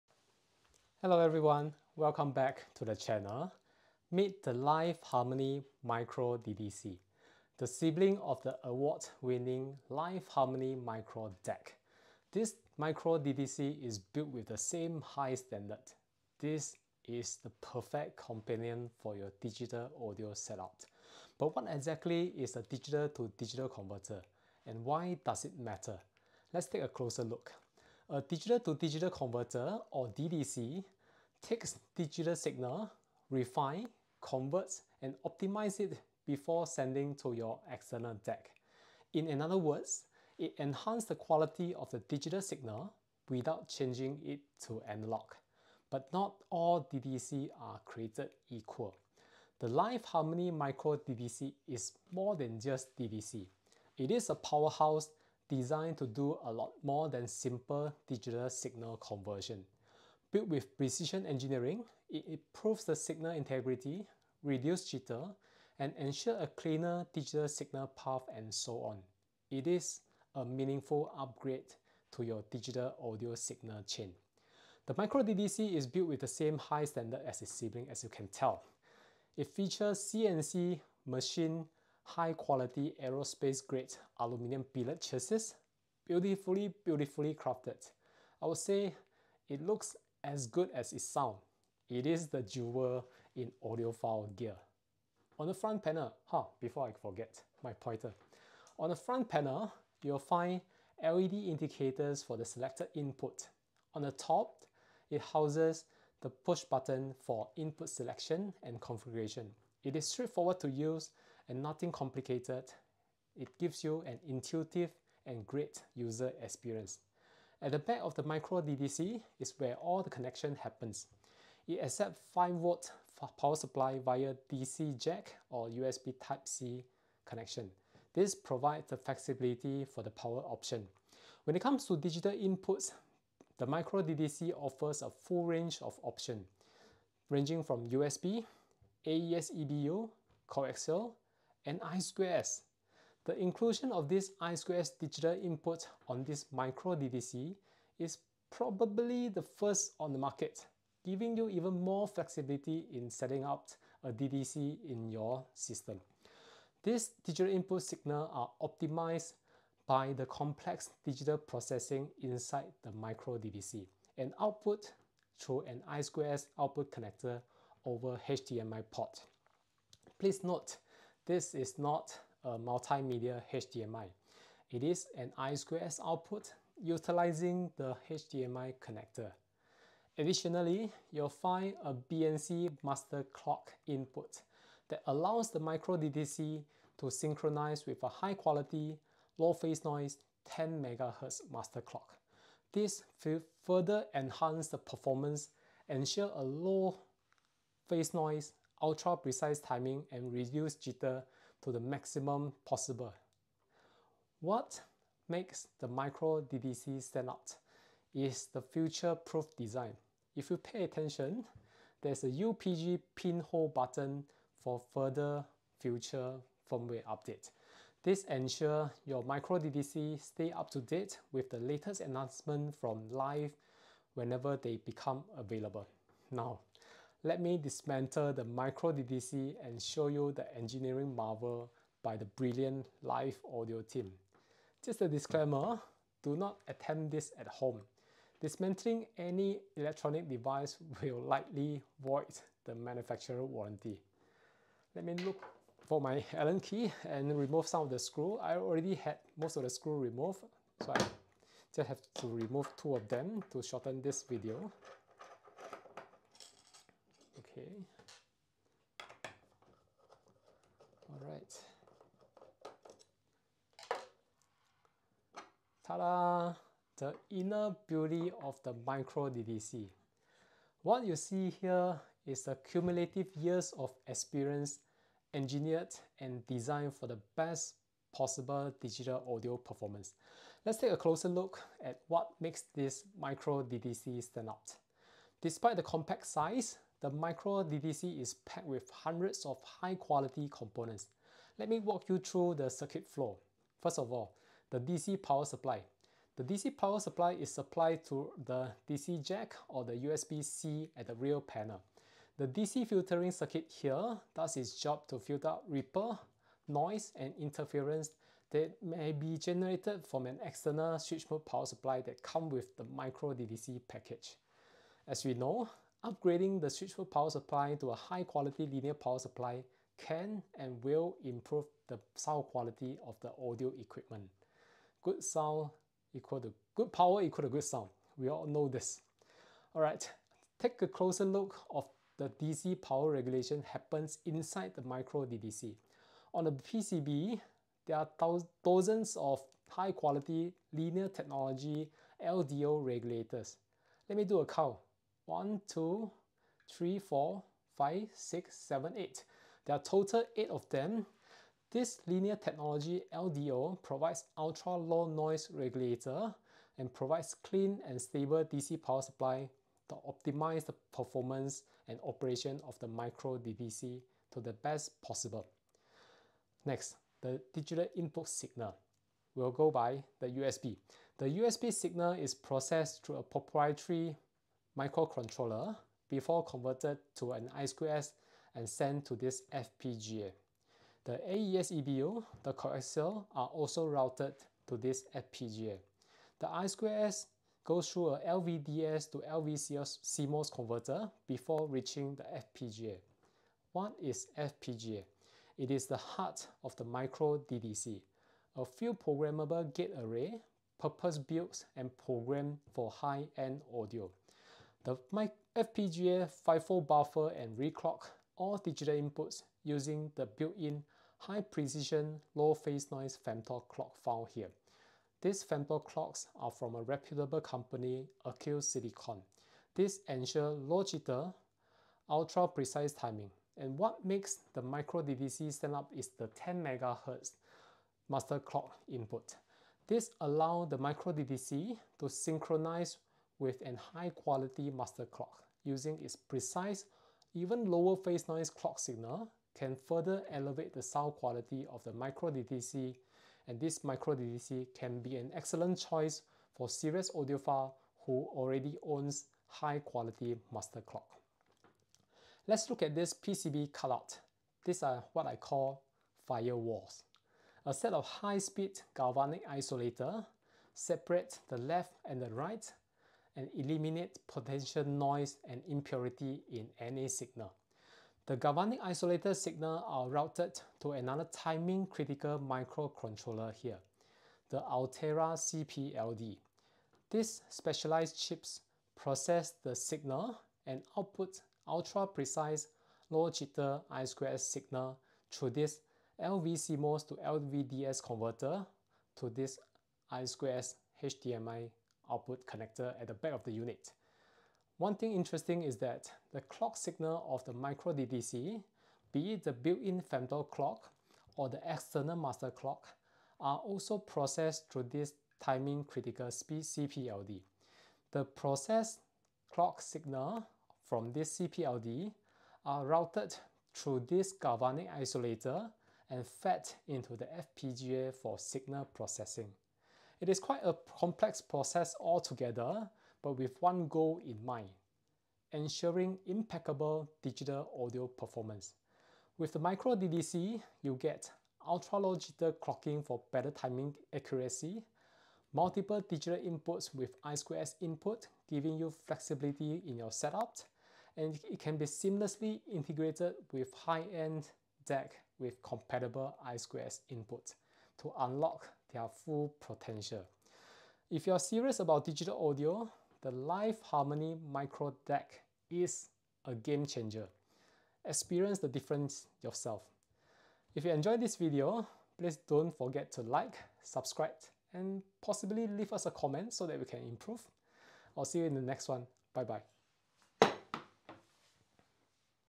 LAiV Harmony µDDC: Inside the Cutting-Edge Digital Audio Architecture
Discover the LAiV Harmony µDDC – a revolutionary Digital-to-Digital Converter (DDC) designed to redefine high-fidelity audio processing. In this video, we take you on an in-depth journey into the anatomy of the uDDC, revealing the advanced engineering that makes it a game-changer in digital audio.